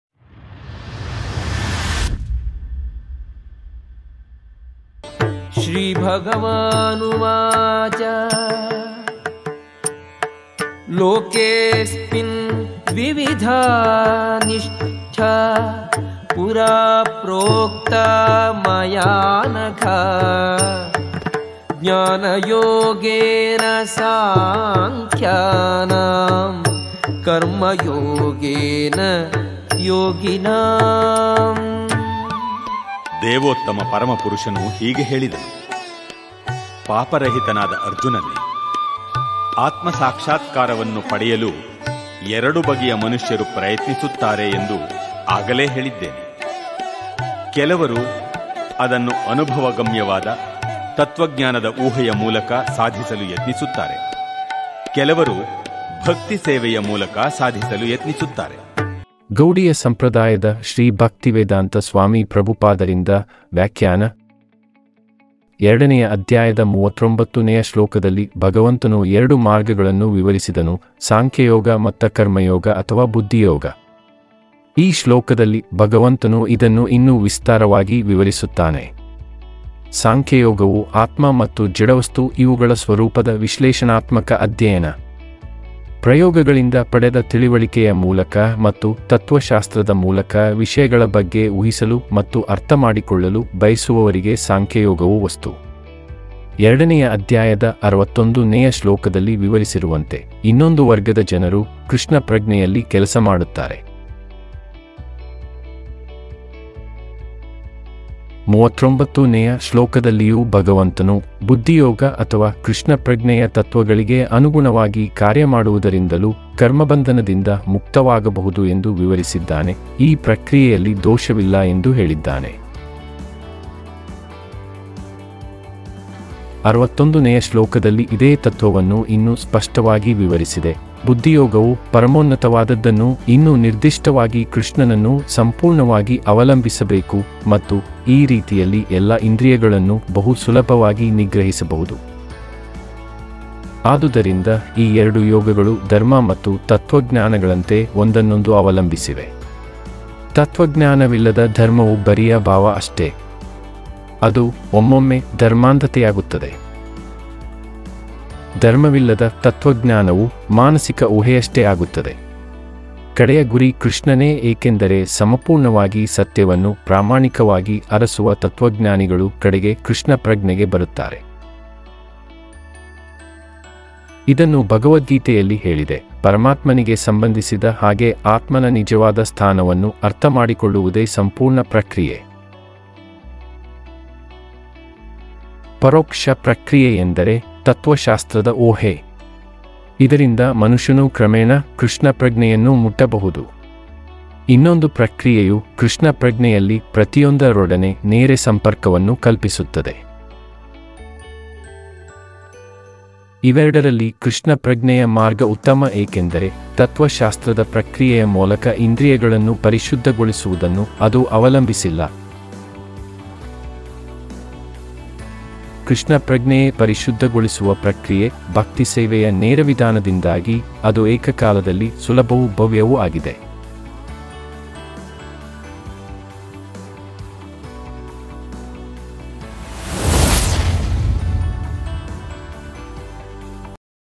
ಭಗವದ್ಗೀತೆ 3.3
ಶ್ರೀ ಭಗವಾನುವಾಚ
ಲೋಕೇsಸ್ಮಿನ್ ದ್ವಿವಿಧಾ ನಿಷ್ಠಾ ಪುರಾಪ್ರೋಕ್ತಾ ಮಯಾನಘ |
ಜ್ಞಾನಯೋಗೇನ ಸಾಂಖ್ಯಾನಾಮ್ ಕರ್ಮಯೋಗೇನ ಯೋಗಿನಾಮ್ ||

ಅನುವಾದ: ದೇವೋತ್ತಮ ಪರಮ ಪುರುಷನು ಹೀಗೆ ಹೇಳಿದನು - ಪಾಪರಹಿತನಾದ ಅರ್ಜುನನೆ, ಆತ್ಮ ಸಾಕ್ಷಾತ್ಕಾರವನ್ನು ಪಡೆಯಲು ಎರಡು ಬಗೆಯ ಮನುಷ್ಯರು ಪ್ರಯತ್ನಿಸುತ್ತಾರೆ ಎಂದು ಆಗಲೇ ಹೇಳಿದ್ದೇನೆ. ಕೆಲವರು ಅದನ್ನು ಅನುಭವಗಮ್ಯವಾದ, ತತ್ವಜ್ಞಾನದ ಊಹೆಯ ಮೂಲಕ ಸಾಧಿಸಲು ಯತ್ನಿಸುತ್ತಾರೆ. ಕೆಲವರು ಭಕ್ತಿಸೇವೆಯ ಮೂಲಕ ಸಾಧಿಸಲು ಯತ್ನಿಸುತ್ತಾರೆ.